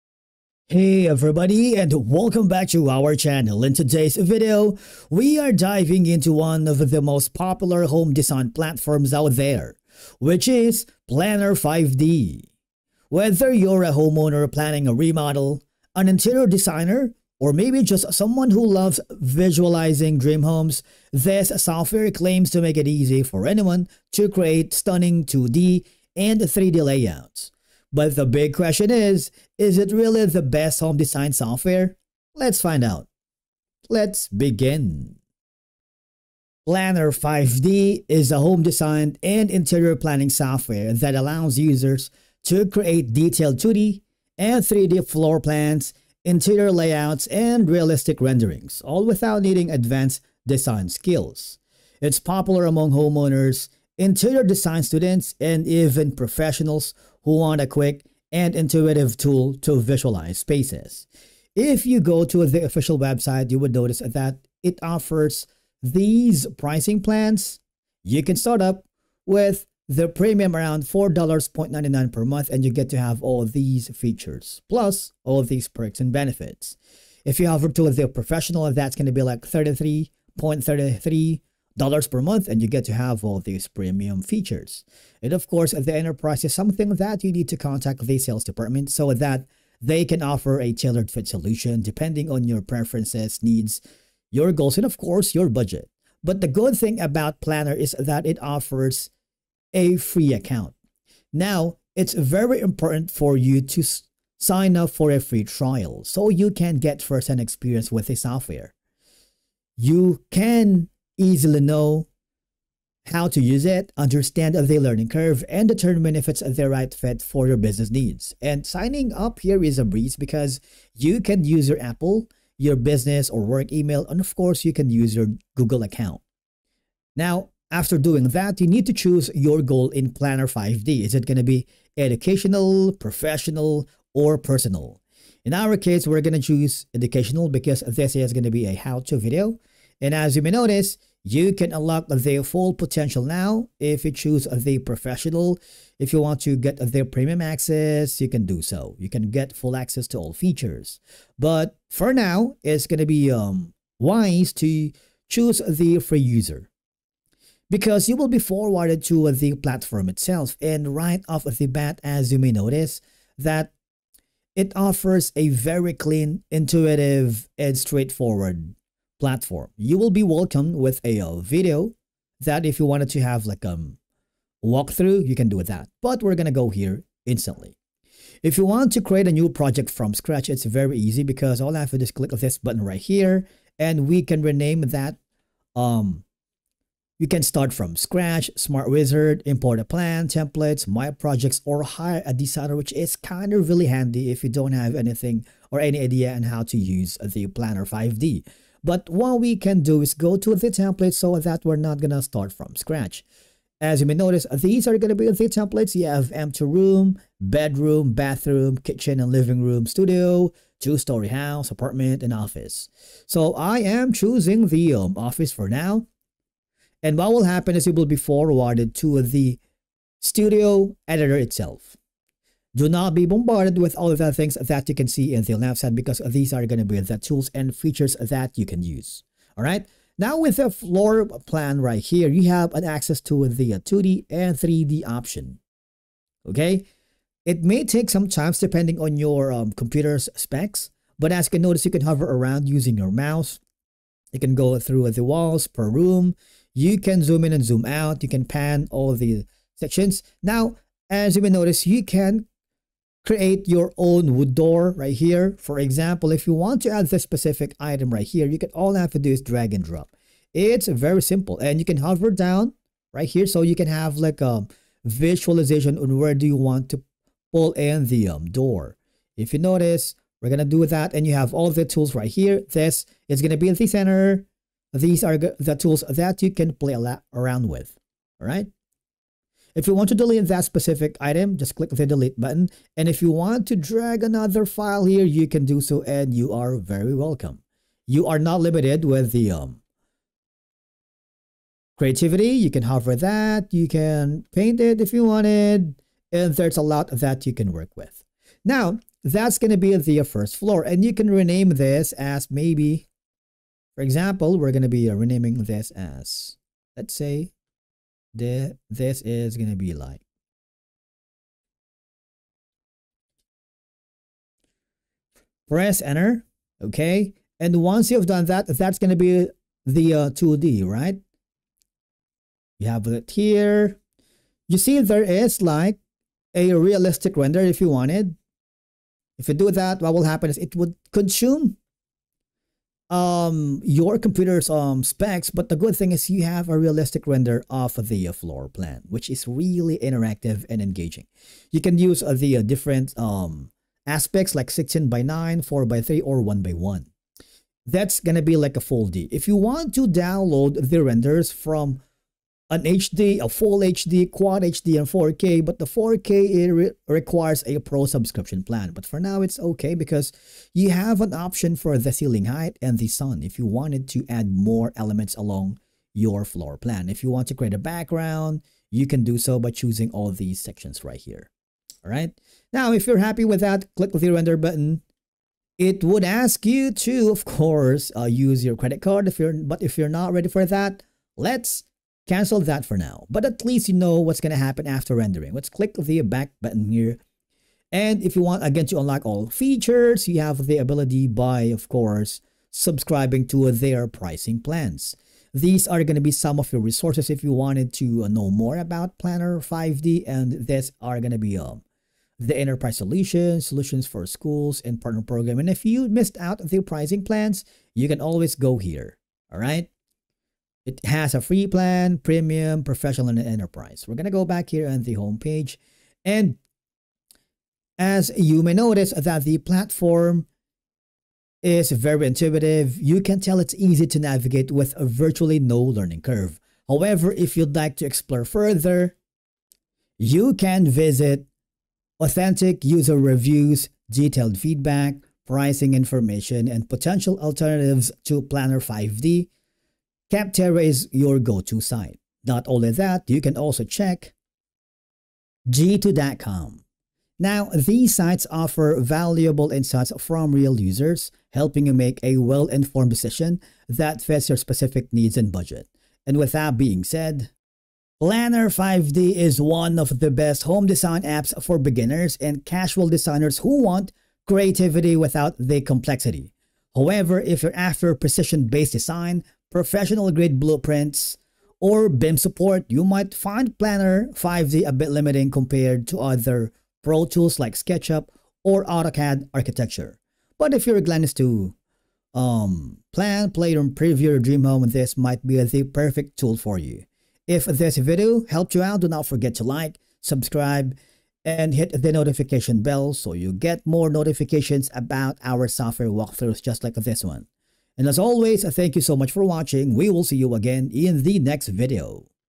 Planner 5D Review & Demo | Best Home Design Software?
In this beginner tutorial and training video, we show you how to install and use Planner 5D software. We show you how to create an account and get started, how to use all of the core features in this demo, and give quick review of the software.